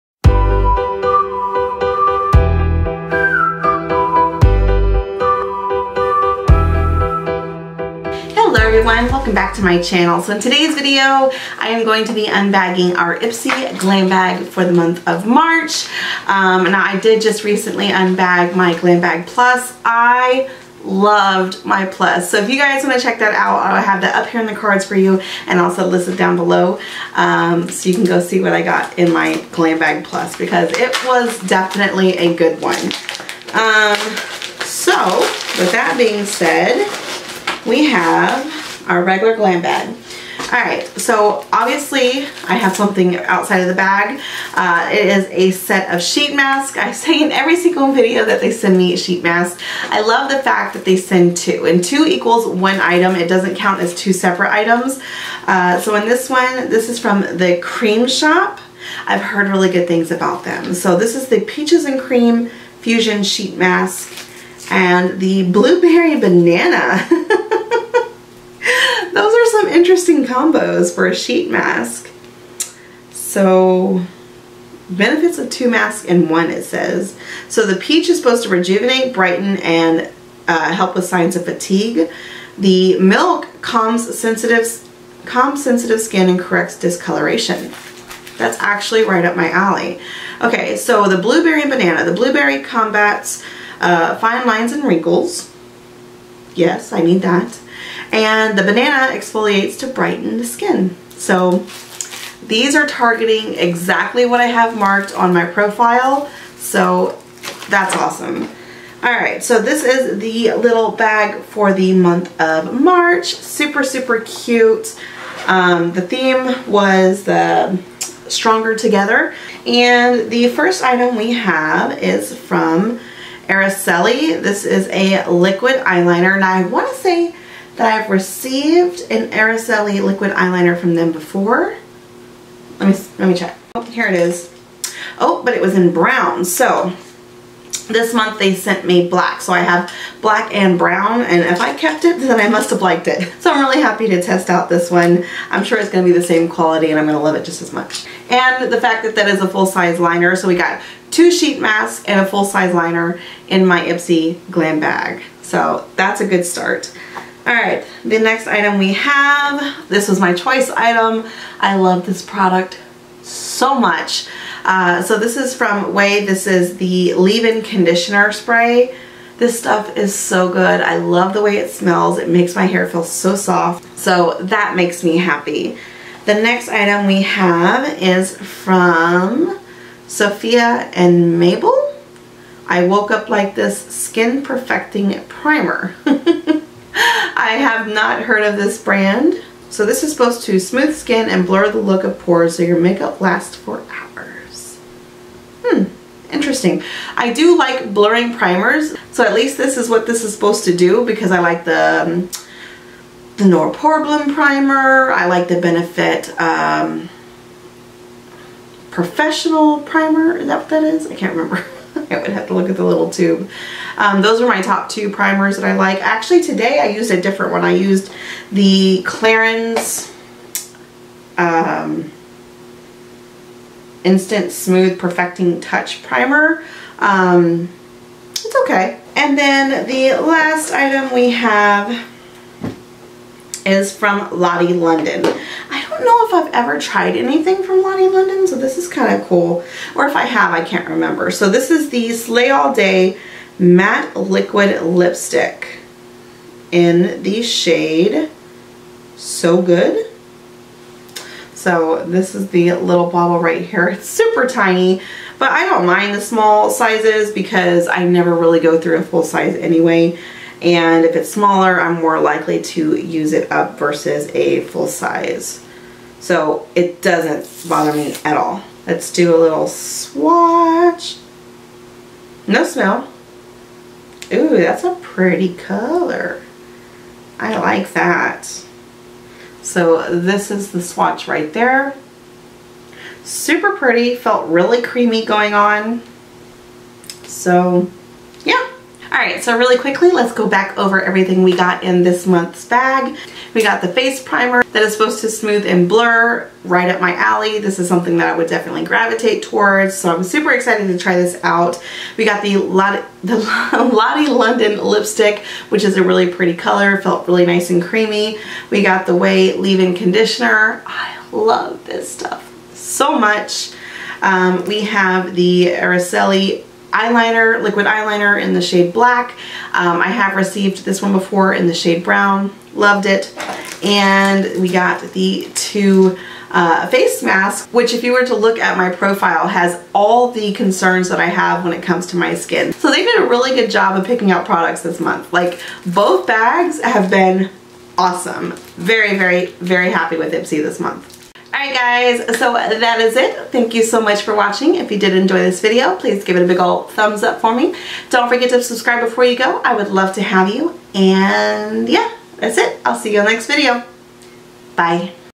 IPSY Glam Bag Unbagging | MARCH 2021
Hey guys! Today's video is an Ipsy Unbagging!! I would love to know what you guys got in your Ipsy bags! XoXo

If you shop online try Rakuten! You get FREE Cash Back!!!  I use it for all my online purchases.

Equipment Used:

Canon 77D Camera